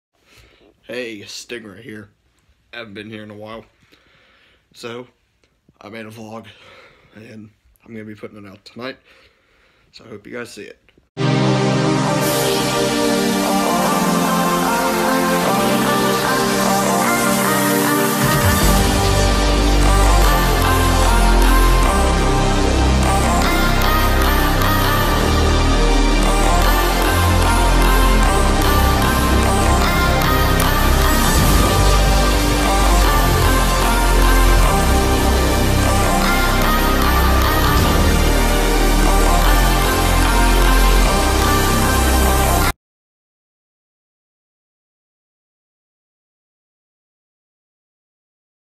Real Life Vlog #2 (Exploring The City) 3-23-19
My phone was about to die when I was vlogging so sorry for it being short but I’m doing a vlog when I’m heading to the beach on April 13th